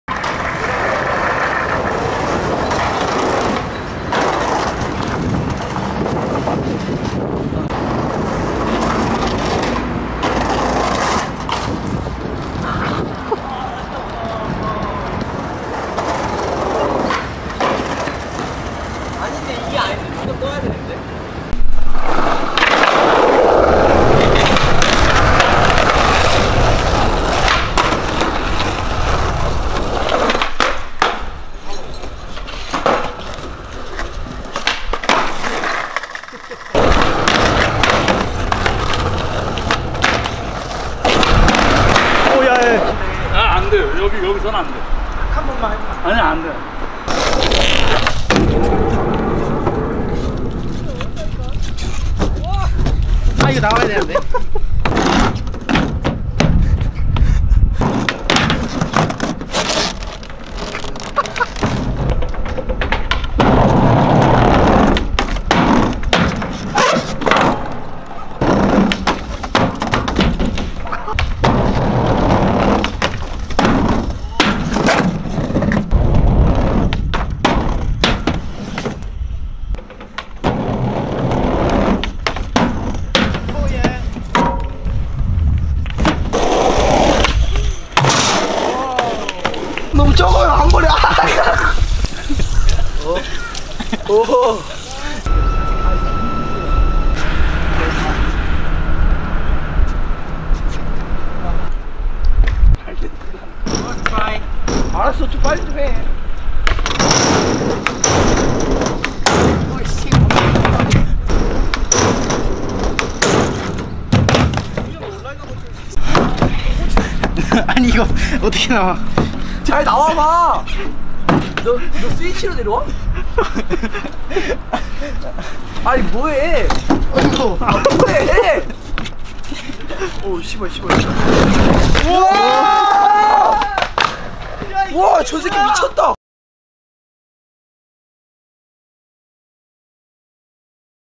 Daily Report: 031 | throwaway [데일리 그라인드 스케이트보드 매거진]
지난 6월 촬영분 중 버리는 클립 모음. 'DAILY MONTAGE 008'도 곧 공개될 예정이니, 많은 기대 바랍니다.

Featuring: 임현성, 유지웅, 최호진, 최유진